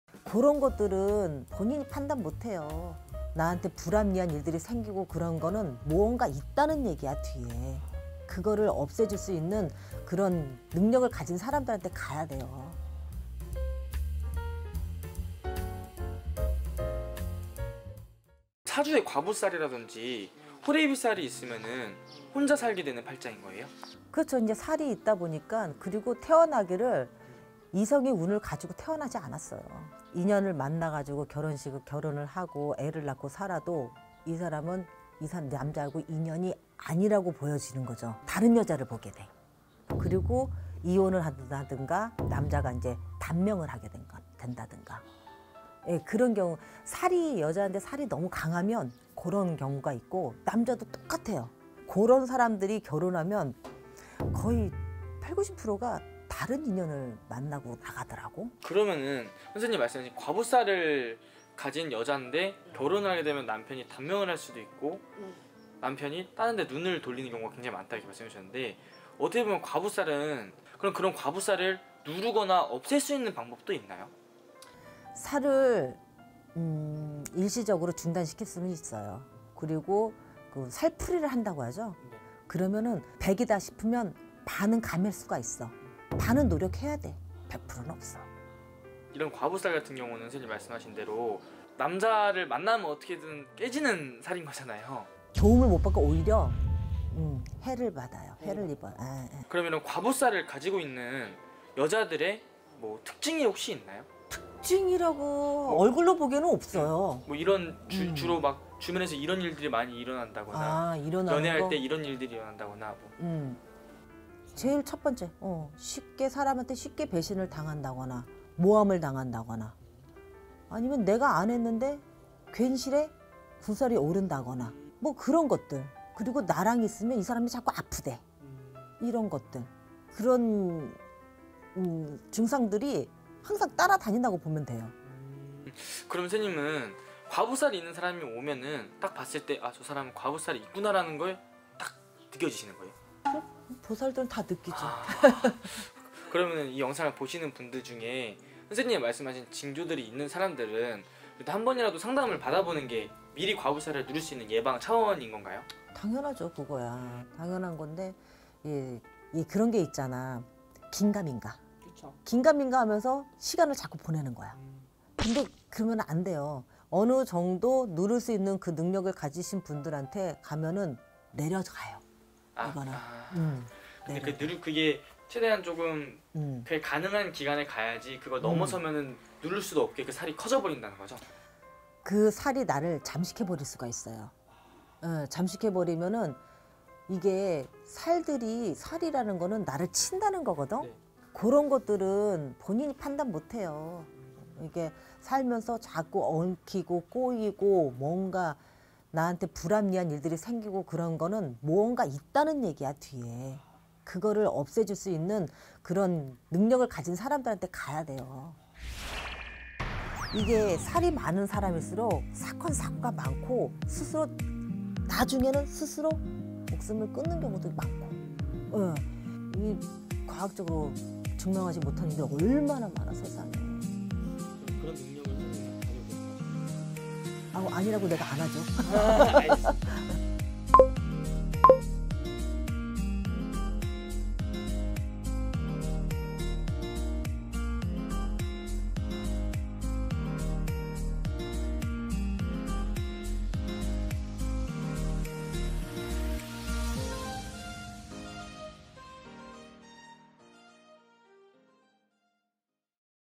과부살을 가지고있는 사람의 특징 / 혼자살아야하는 팔자를 가진 박복한 여성사주의 특징 [포천점집 수정보살]
자막

★대표블로그★
-올마이티TV